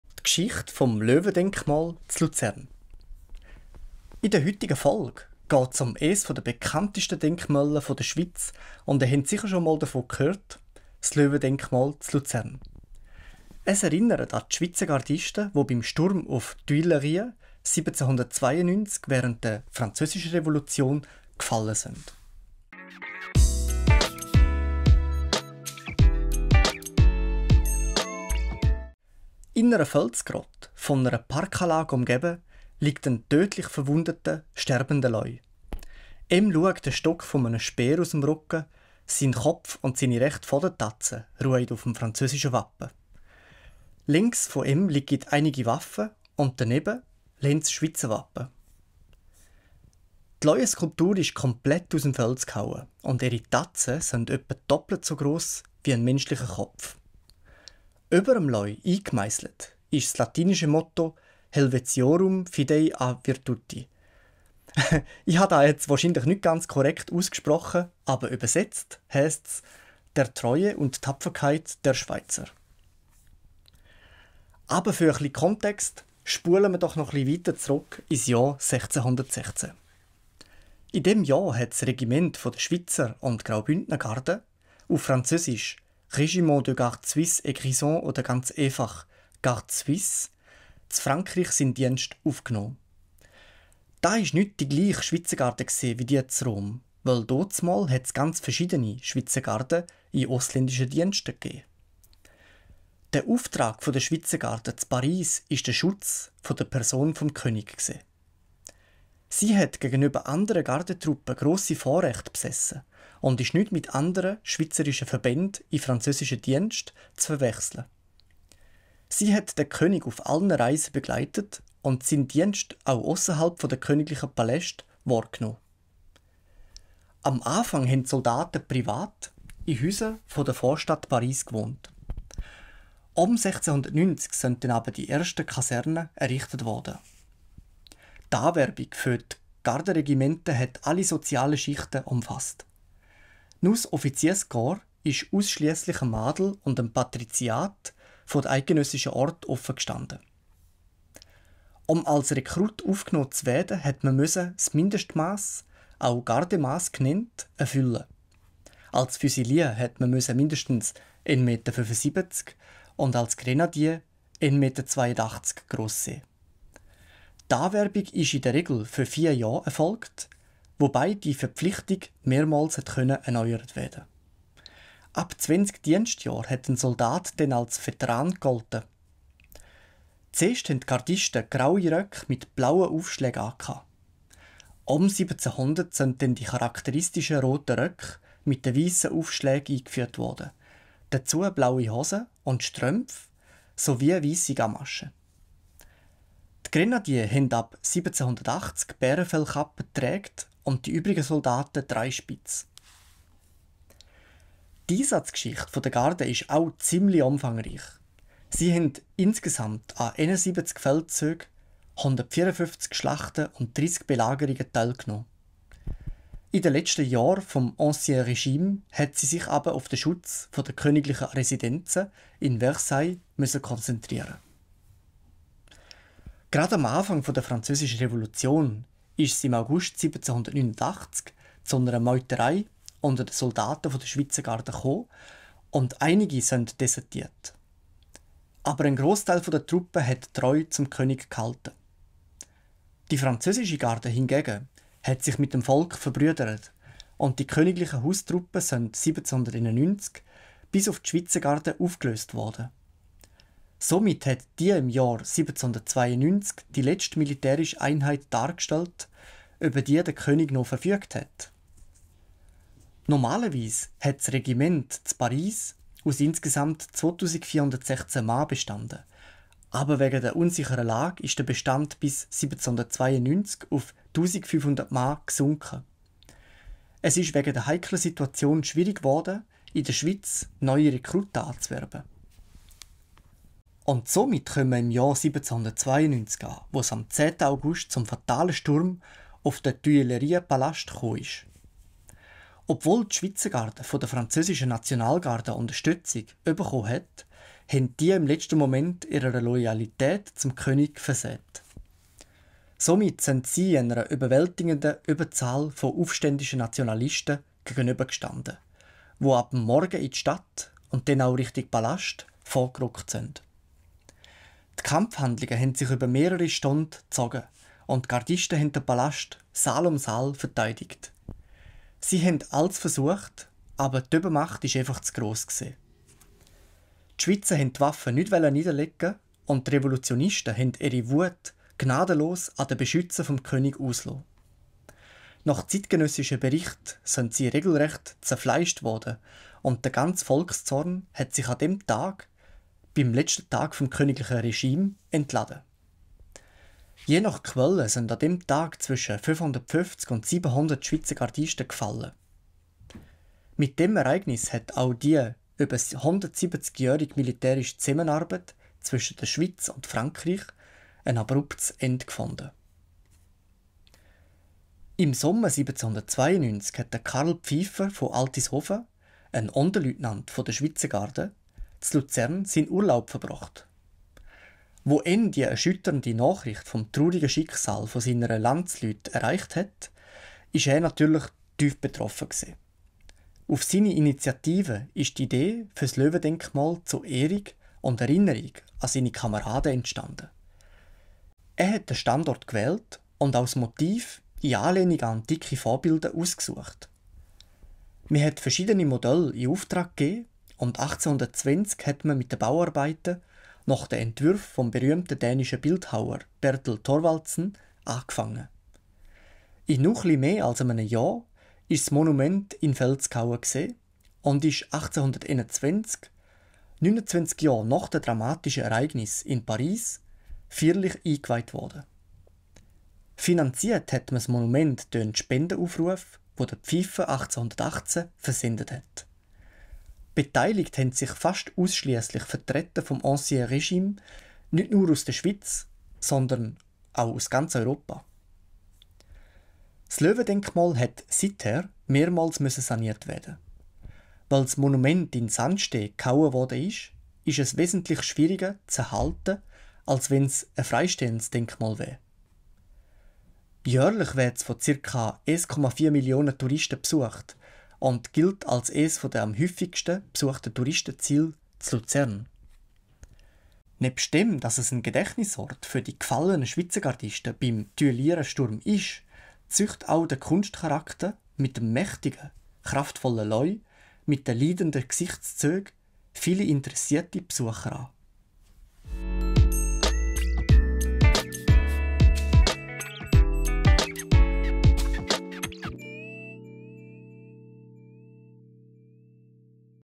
Das Löwendenkmal Luzern und seine Geschichte - Ein Podcast zum Schweizerdeutsch Lernen
🇨🇭ONLINE LEKTIONEN: Bist du interessiert an 1:1 Lektionen für Schweizerdeutsch?